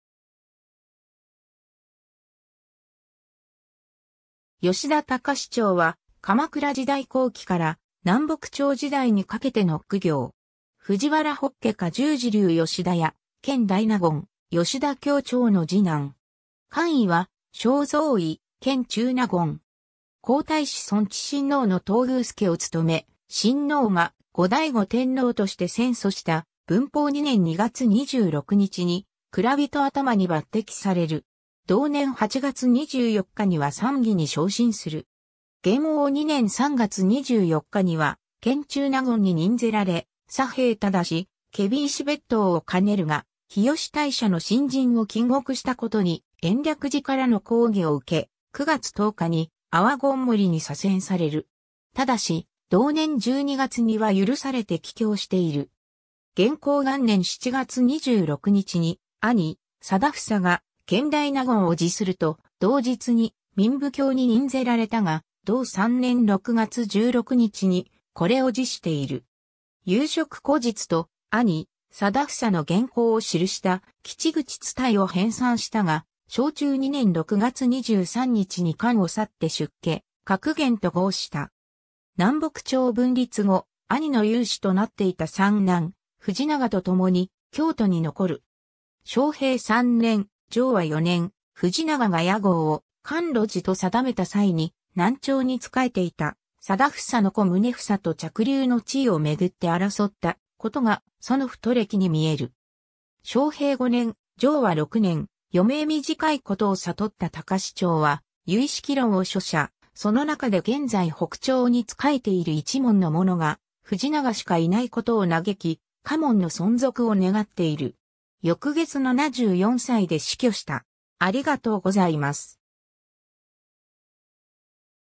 吉田隆長
吉田 隆長（よしだ たかなが）は、鎌倉時代後期から南北朝時代にかけての公卿。
藤原北家勧修寺流吉田家、権大納言・吉田経長の次男。
官位は正三位・権中納言。
 皇太子尊治親王の東宮亮を務め、親王が後醍醐天皇として践祚した文保2年（1318年）2月26日に蔵人頭に抜擢される。
同年8月24日には参議に昇進する。
元応2年（1320年）3月24日には権中納言に任ぜられ、左兵衛督・検非違使別当を兼ねるが、日吉大社の神人を禁獄したことに延暦寺からの抗議を受け、9月10日に阿波権守に左遷される。
ただし、同年12月には許されて帰京している。
 元亨元年（1321年）7月26日に兄・定房が権大納言を辞すると、同日に民部卿に任ぜられたが同3年6月16日にこれを辞している。
有職故実と兄・定房の言行を記した『吉口伝』（『夕郎故実』）を編纂したが、正中2年（1325年）6月23日に官を去って出家、覚源と号した。
 南北朝分立後、兄の猶子となっていた三男・藤長と共に京都に残る。
正平3年/貞和4年（1348年）、藤長が家号を「甘露寺」と定めた際に、南朝に仕えていた定房の子宗房（後の南朝右大臣）と嫡流の地位を巡って争ったことが『園太暦』に見える。
 正平5年/貞和6年（1350年）、余命短い事を悟った隆長は『唯識論』を書写、その中で現在北朝に仕えている一門の者が藤長しかいないことを嘆き、家門の存続を願っている。
翌月74歳で死去した（『園太暦』）。